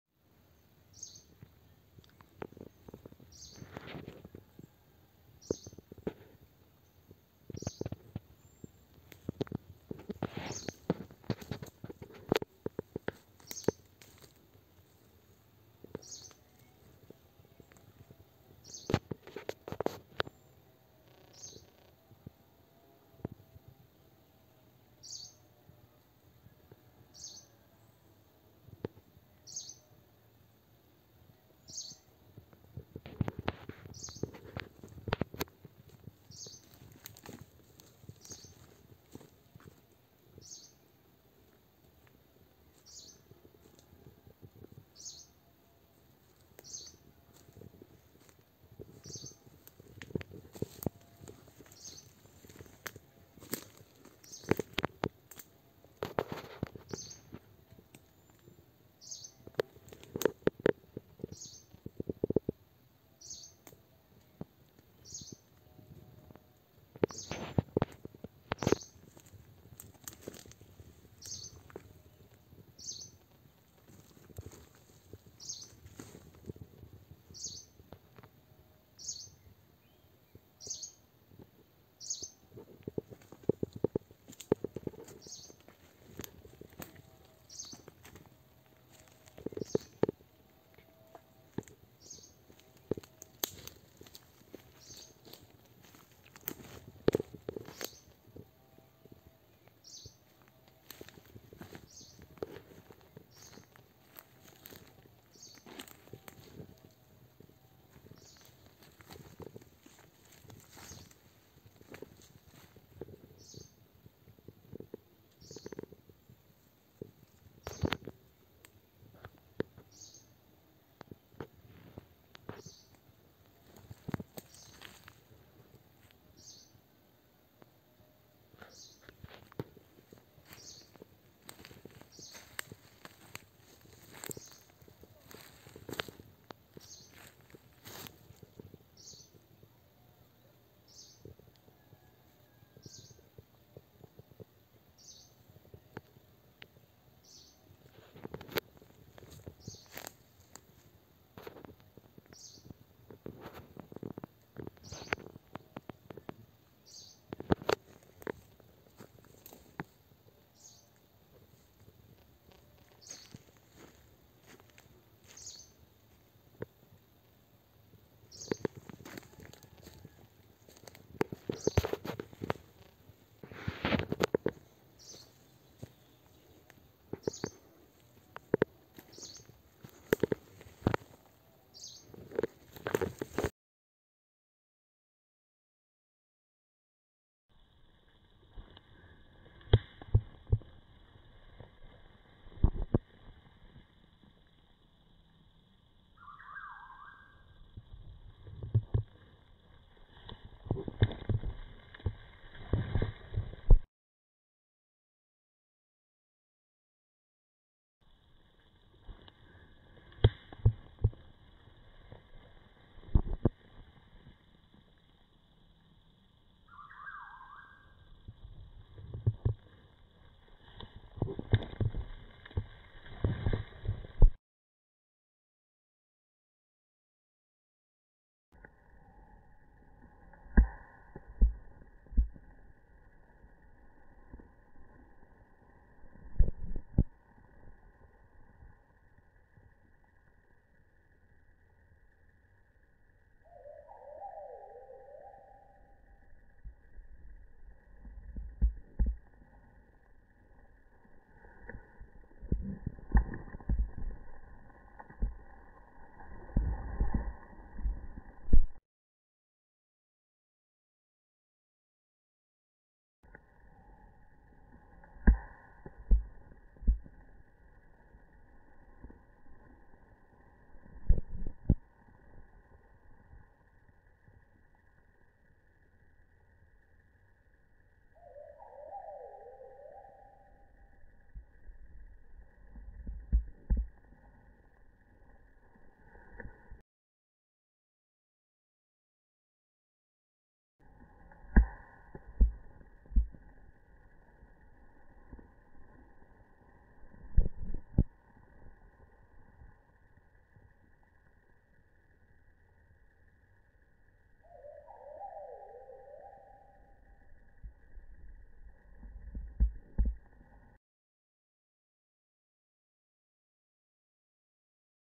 Strange creatures scare people in the woods. humanoid. cryptids
Paranormal Strange creatures scare people in the woods. humanoid hiding behind a tree and wanted to attack the operator. cryptids they live in deep forests and hide from people when they are seen. this unknown creature looked like an alien. how it appeared in the woods, maybe a UFO crashed in the woods and the aliens survived by a miracle. mysterious creatures, dangerous can attack people. recently, people in the forests have begun to see scary creatures